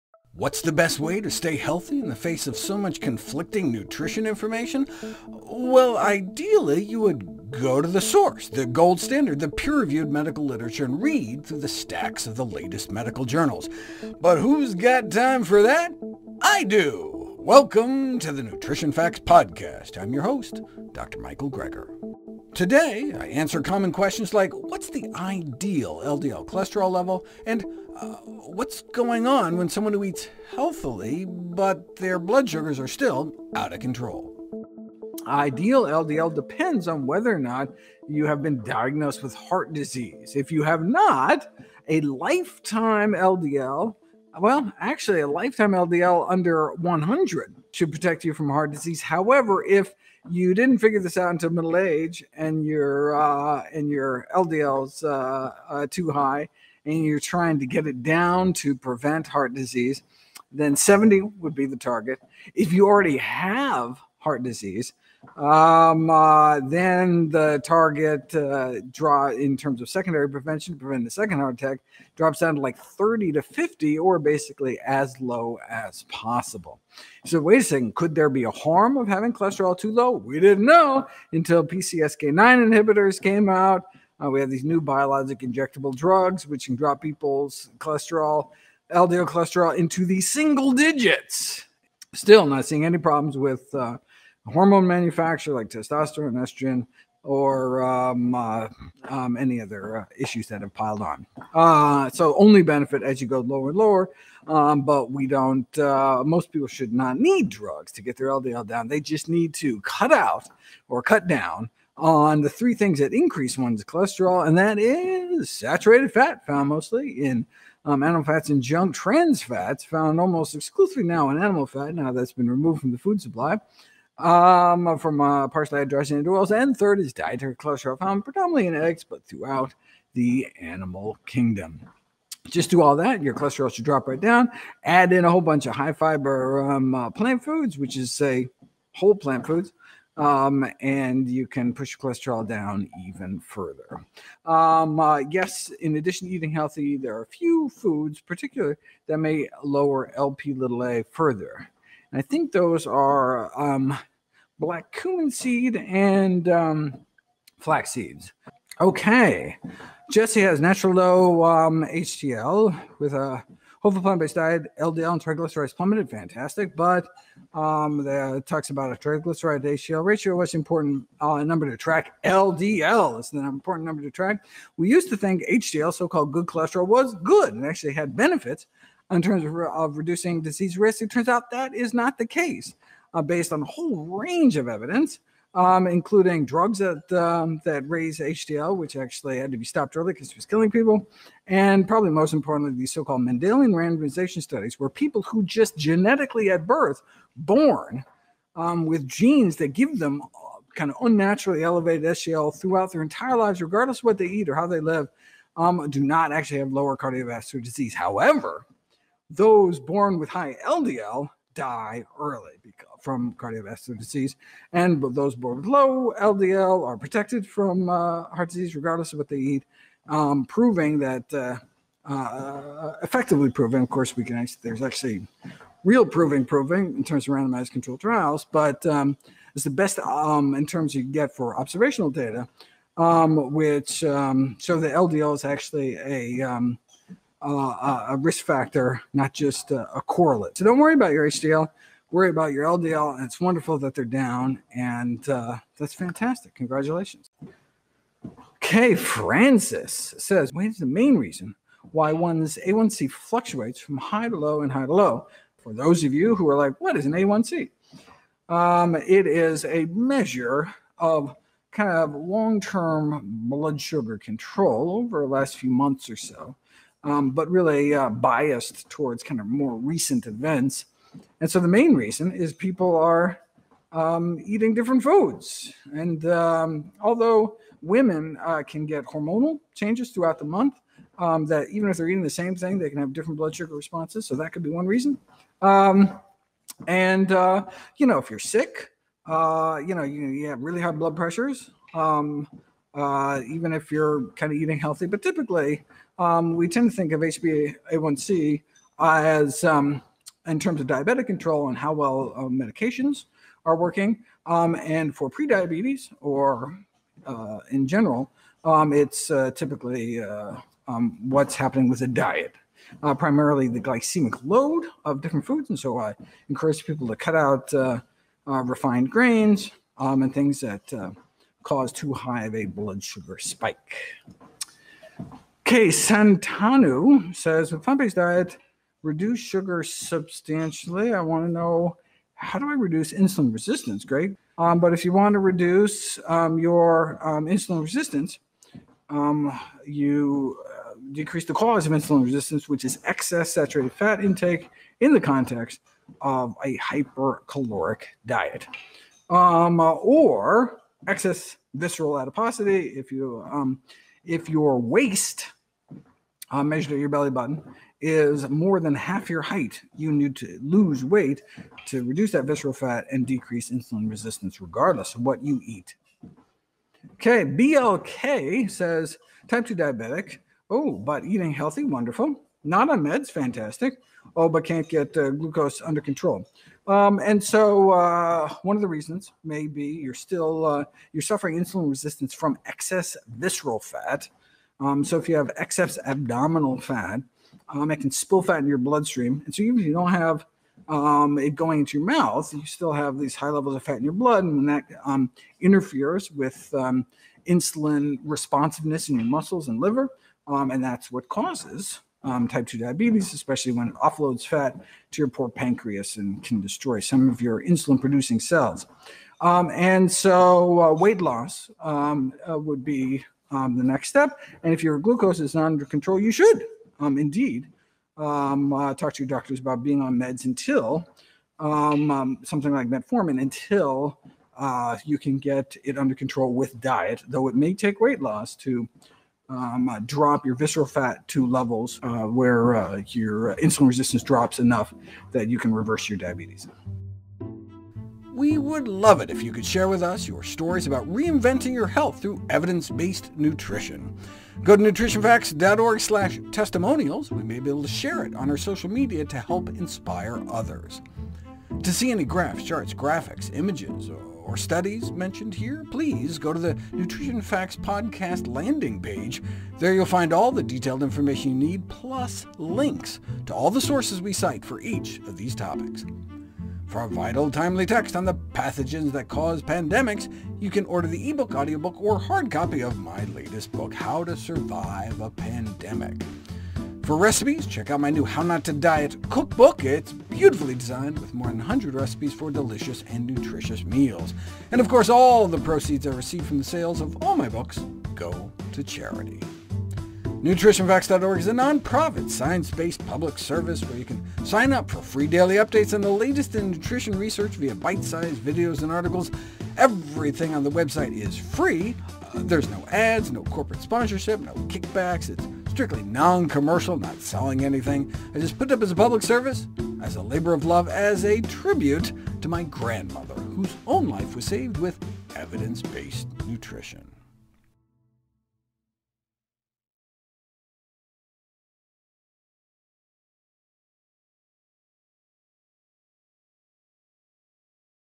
Podcast: Cholesterol and Diabetes Q&A
There is no shortage of questions about these two increasingly common medical conditions: high cholesterol and diabetes.